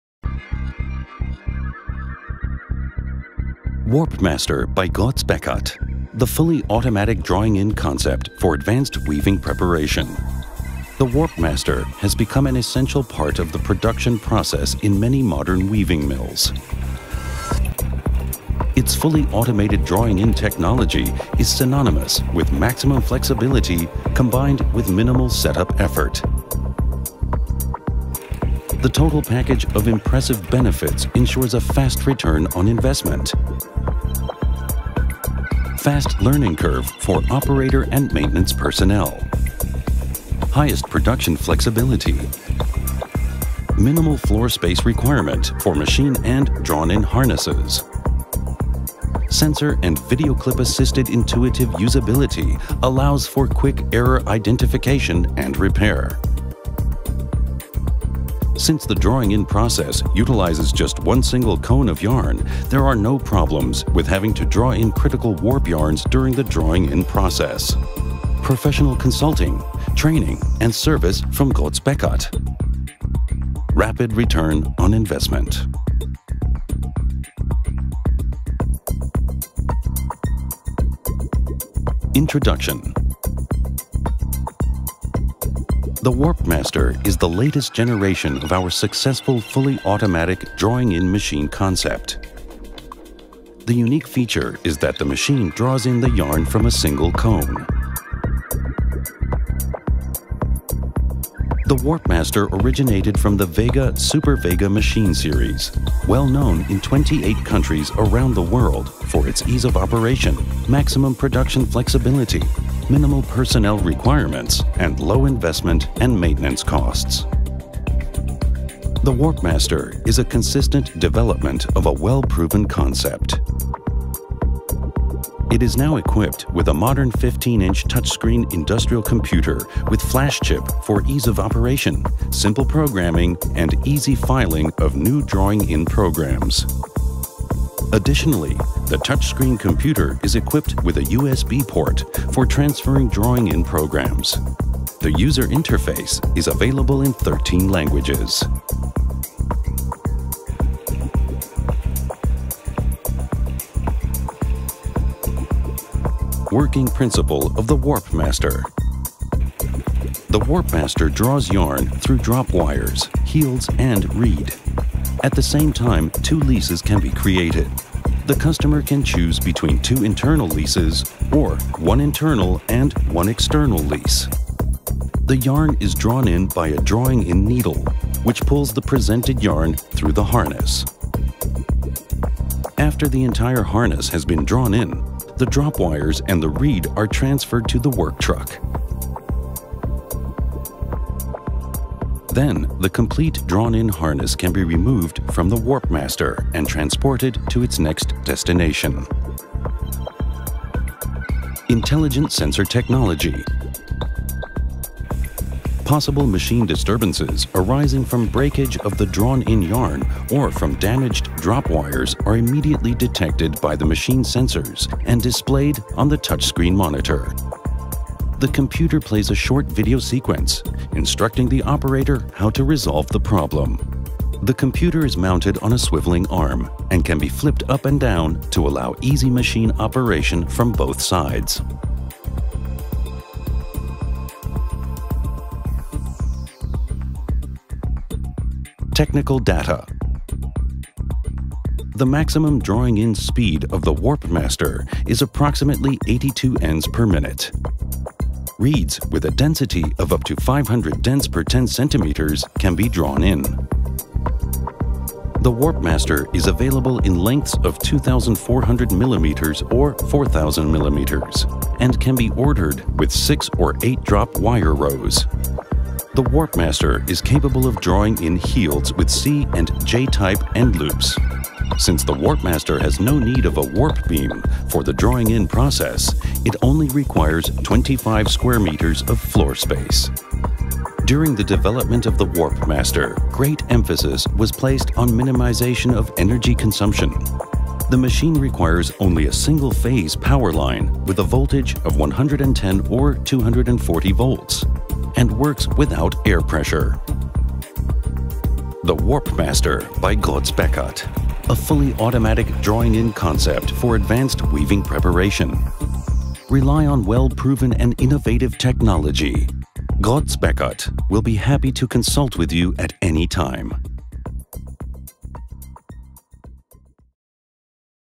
Weaving Preparation: The WarpMaster
The drawing-in concept by Groz-Beckert is proven in the field of weaving preparation all over the world and impresses with easy operation and conversion: As shown in the video, with the WarpMaster the yarn is drawn in from a single bobbin and threaded through the drop wire, heald and reed and is thus independent from the warp beam. This ensures maximum flexibility with minimal setup requirements. The small space requirements for the machine and harness as well as the low energy consumption and simple, digital control are additional advantages offered by the WarpMaster.